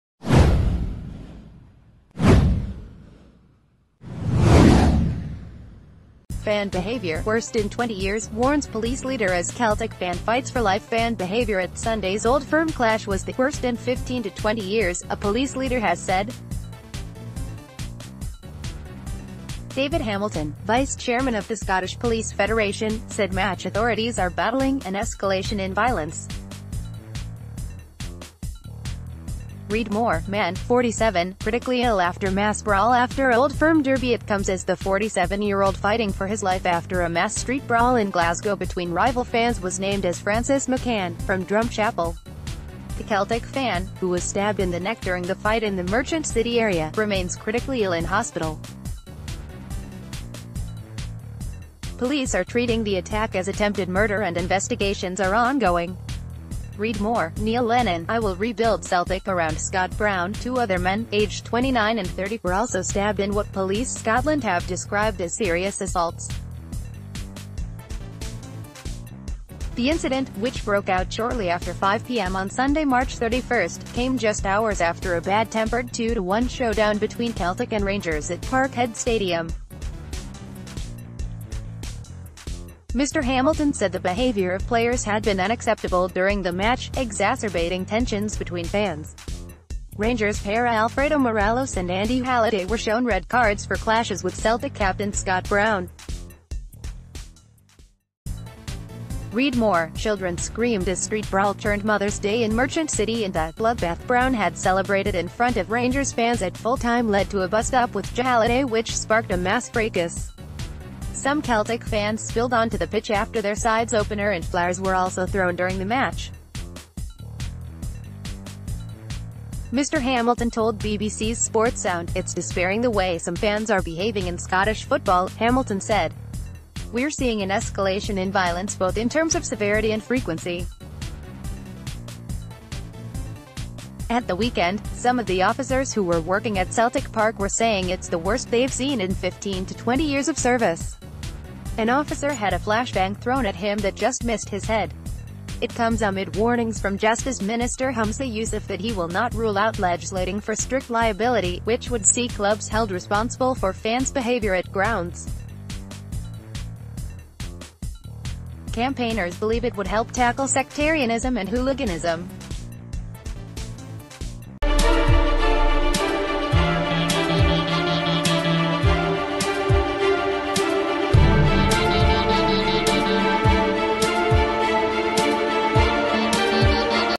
Fan behaviour 'worst in 20 years' warns police leader as Celtic fan fights for life
Fan behaviour 'worst in 20 years' warns police leader as Celtic fan fights for life FAN behaviour at Sunday's Old Firm clash was the "worst in 15 to 20 years", a police leader has said. David Hamilton, vice-chairman of the Scottish Police Federation, said match authorities are battling "an escalation in violence". Read more: Man, 47, critically ill after mass brawl after Old Firm derby It comes as the 47-year-old fighting for his life after a mass street brawl in Glasgow between rival fans was n...